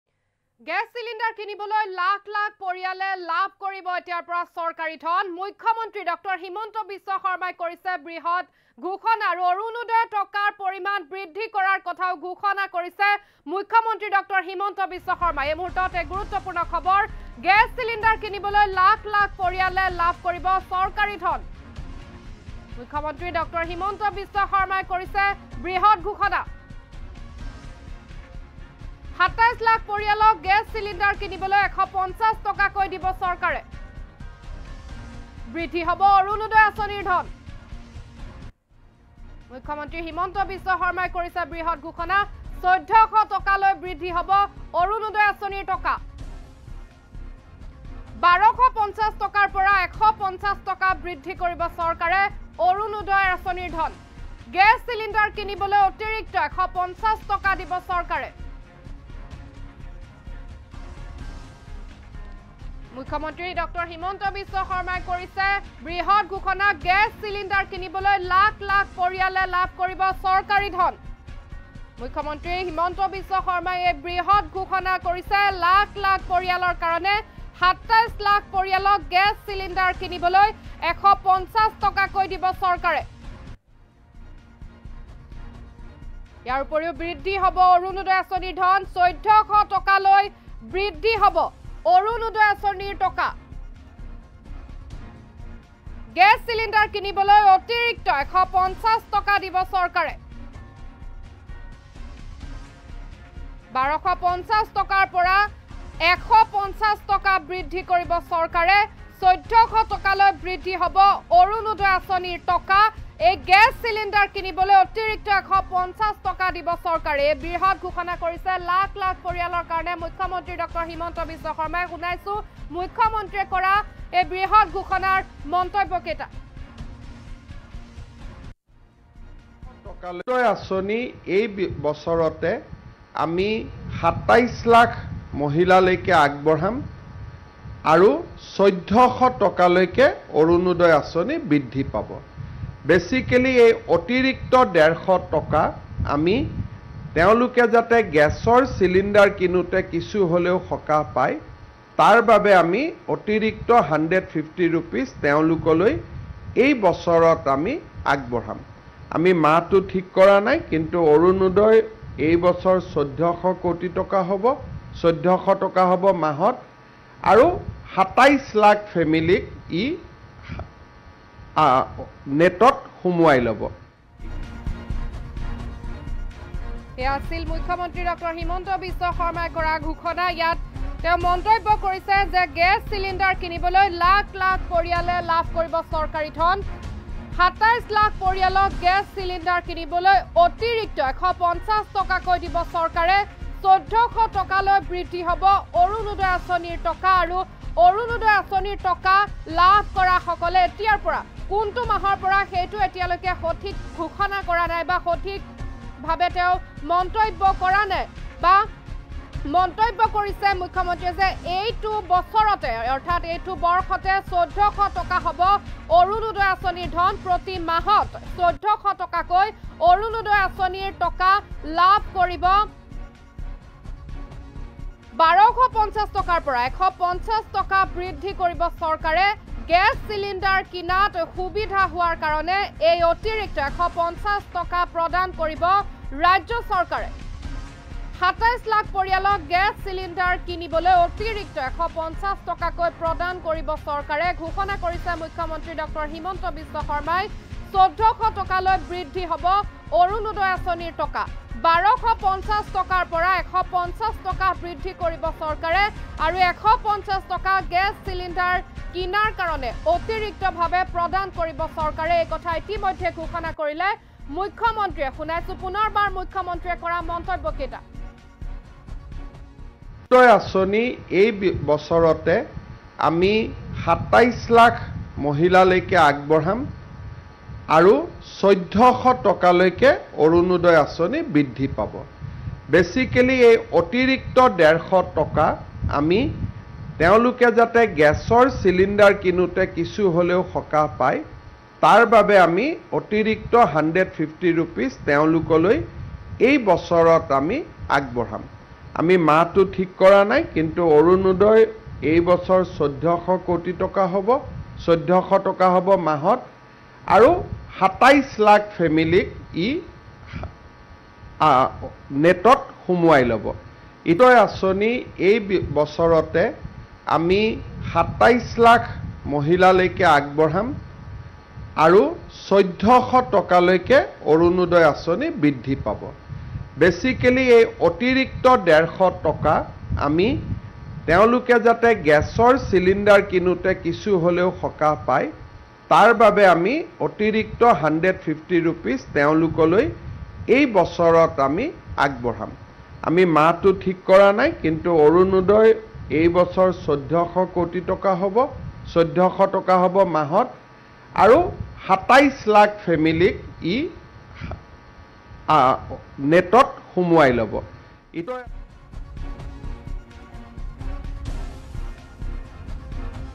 গেছ চিলিণ্ডাৰ কিনিবলৈ লাখ লাখ পৰিয়ালে লাভ কৰিব চৰকাৰী ধন।
গেছ চিলিণ্ডাৰ কিনিবলৈ লাখ লাখ পৰিয়ালে লাভ কৰিব চৰকাৰী ধন। মুখ্যমন্ত্ৰী হিমন্ত বিশ্ব শৰ্মাৰ বৃহত্‍ ঘোষণা। কোনে কোনে লাভ কৰিব এই আঁচনিৰ সুবিধা?